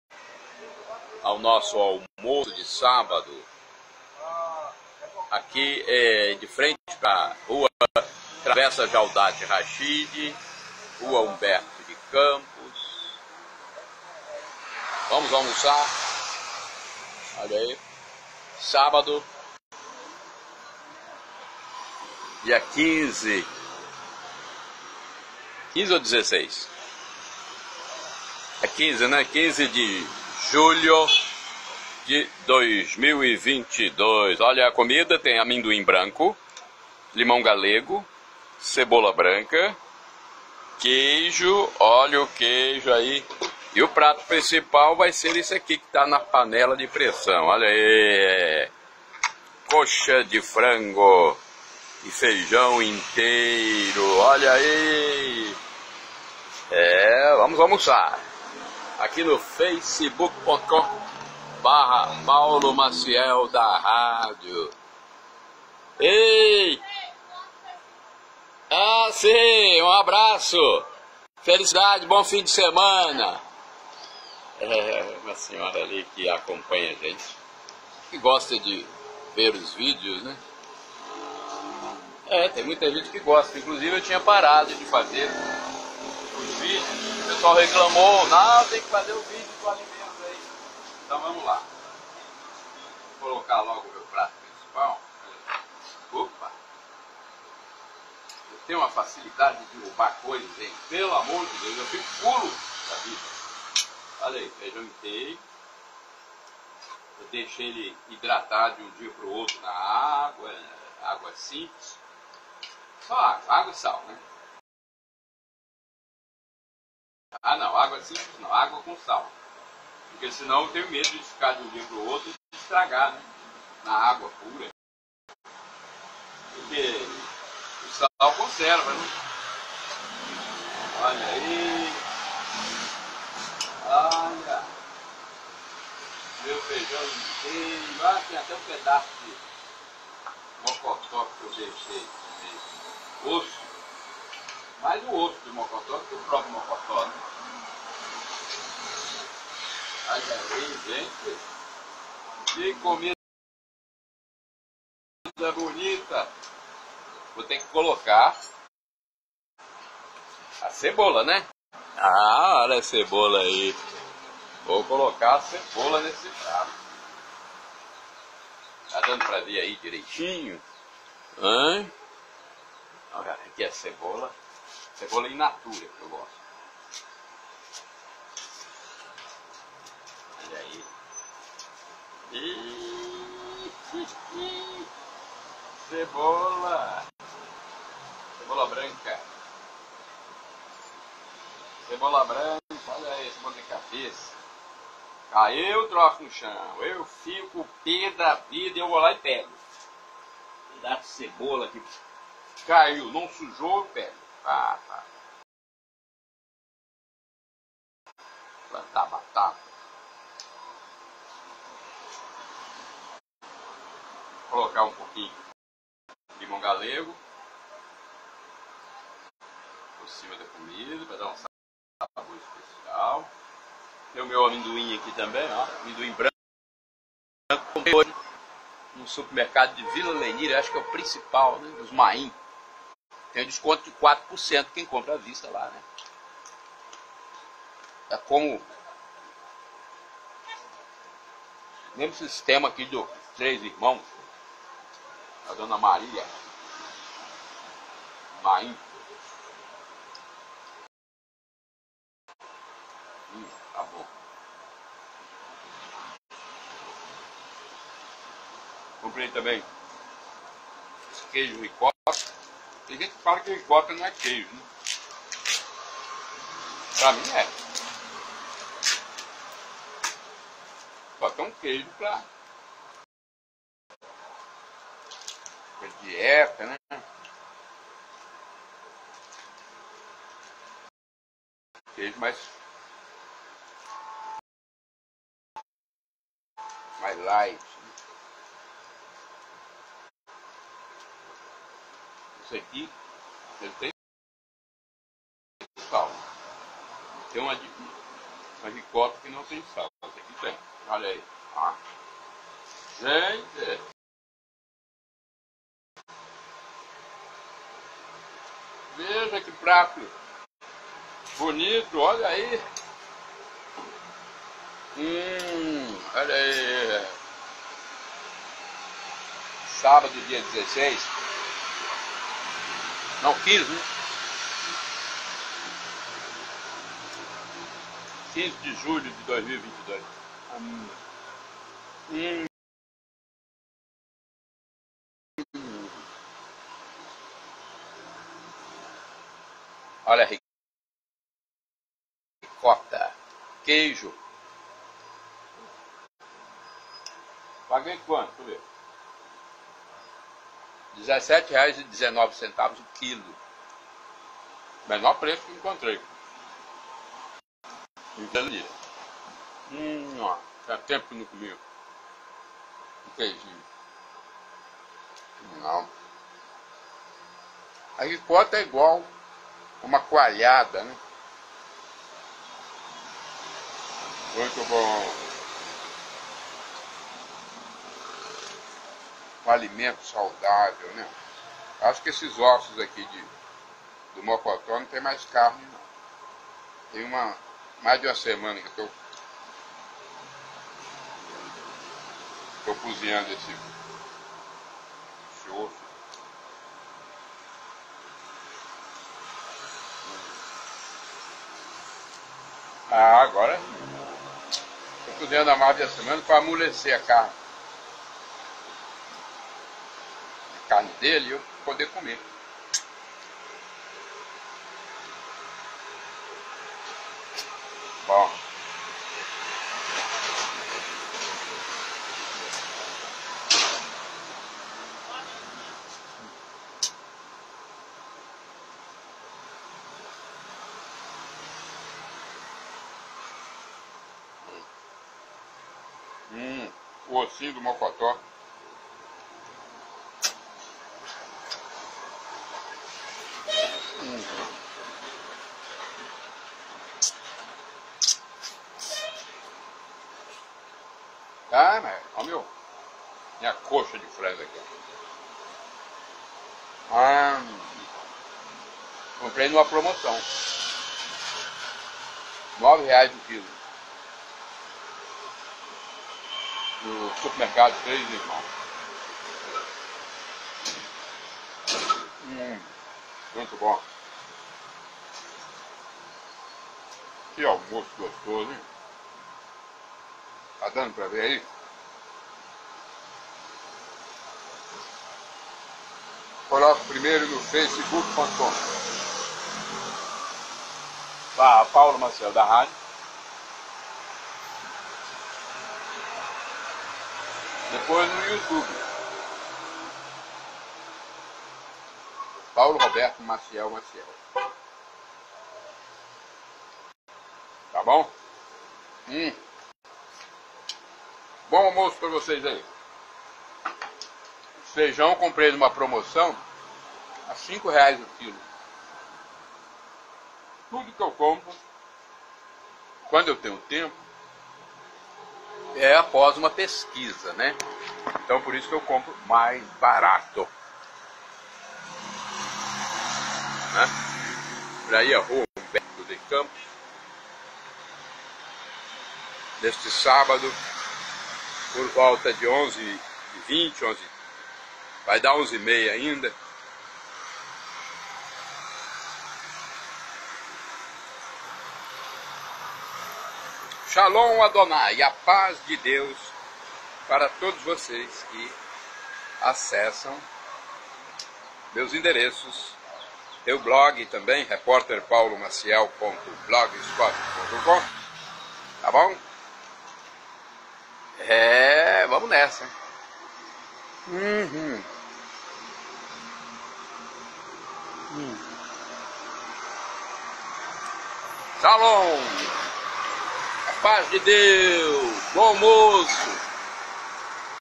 ALMOÇO. Feijão, coxa de frango, amendoim, osso. Por repórter Paulo Maciel. Colatina/ES, 16/07/2022
ALMOÇO: Feijão, coxa de ave, amendoim, queijo. Repórter Paulo Maciel. Colatina/ES (16/07/2022)
---TEXTO, filmagem e narração: REPÓRTER/RADIALISTA Paulo Maciel, de Colatina/ES, no CANAL DO YOUTUBE "Paulo Roberto Maciel"; " e BLOG "reporterpaulomaciel.blogspot.com"
-- Feijão carioca inteiro cozido em panela de pressão, coxa de ave (frango), osso de mocotó bovino, amendoim branco in natura, queijo (ricota), cebola de cabeça (branca), limão galego. Este é o almoço de sexta-feira (15/07/2022) do repórter Paulo Maciel, em Colatina, município da região noroeste do estado do Espírito Santo com mais de 124 mil habitantes (Censo 2021).
--A refeição foi saboreada na varanda da casa, de frente para a rua Humberto de Campos e travessa Jaudat Rachid, vias de acesso à cidade alta, ou seja, os bairros Nossa Senhora do Perpétuo Socorro, Operário, Pôr do Sol, Moacir Brotas, Jardim Planalto, Vista da Serra e Residencial Nobre.